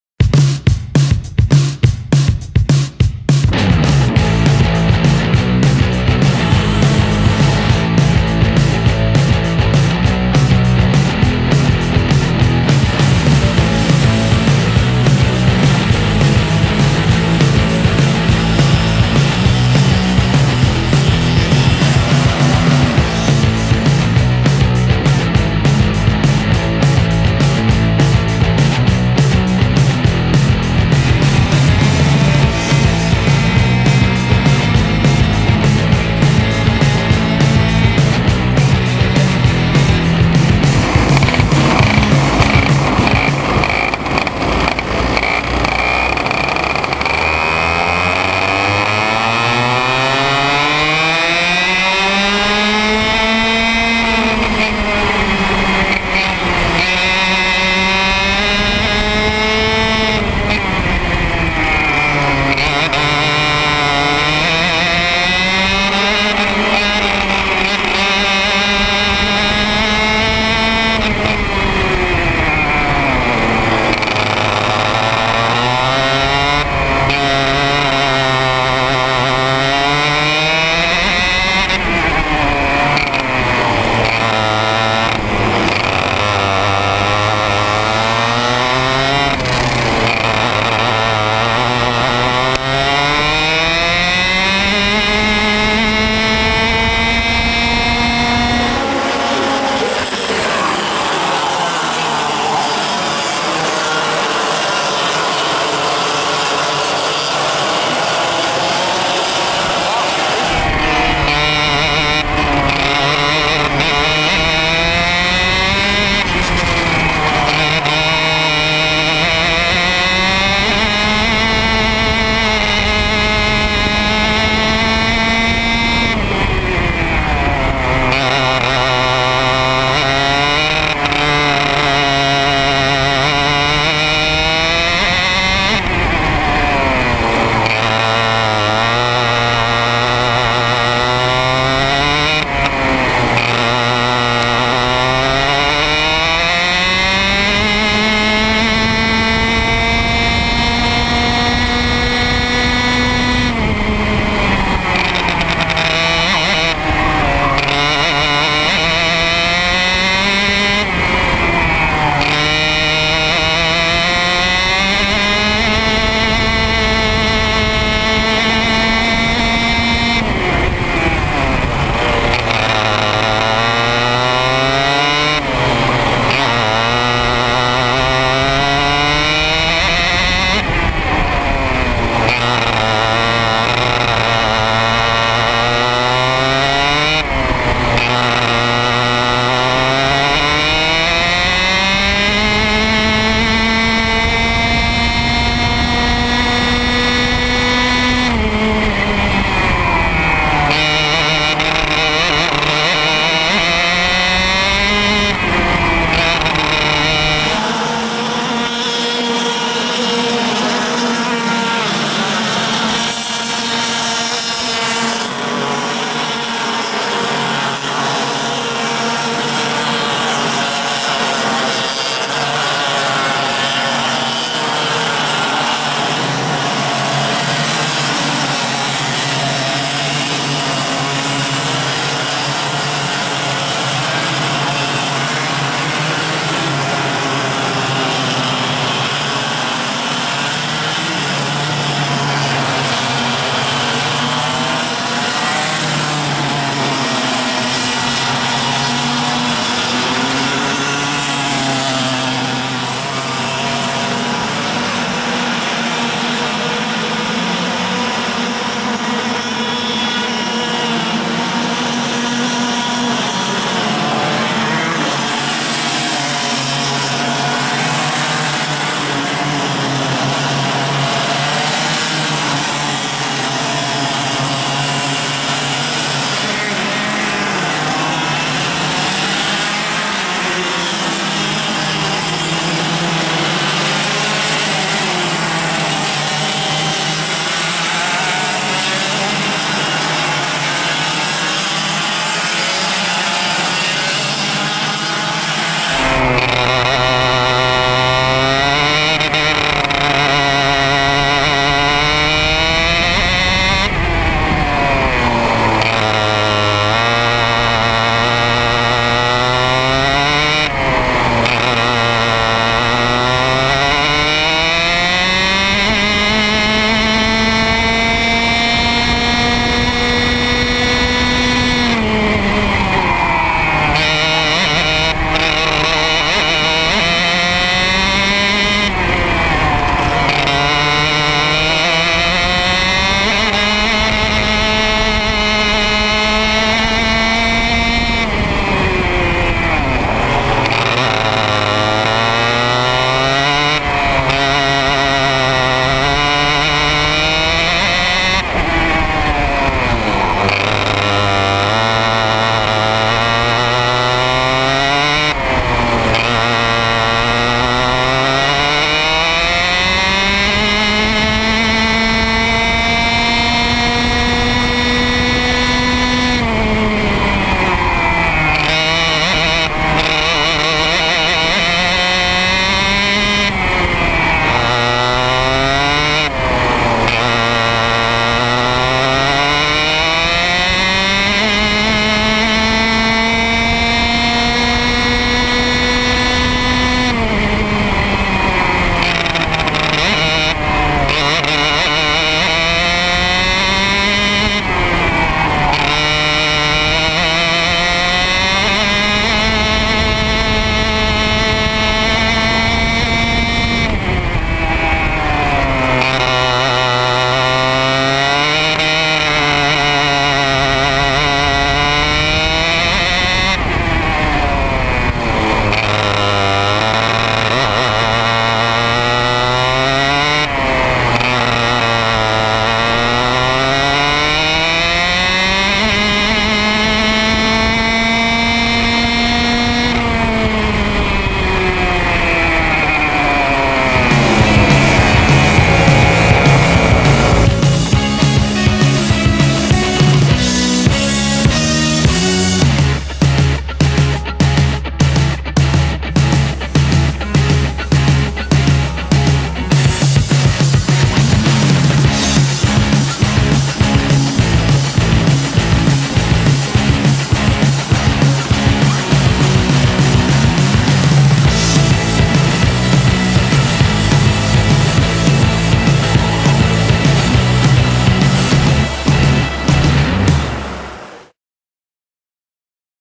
2ª Et. Copa São Paulo Light de Kart 2017 - #35 Pedro Aizza /Junior Menor 1ª prova
VÍdeo on-board do piloto Pedro Aizza na primeira prova da categoria Junior Menor durante a 2ª Etapa da Copa São Paulo Light de Kart que foi realizado no dia 04 de Março no Kartódromo Aldeia da Serra  - Barueri/Brasil.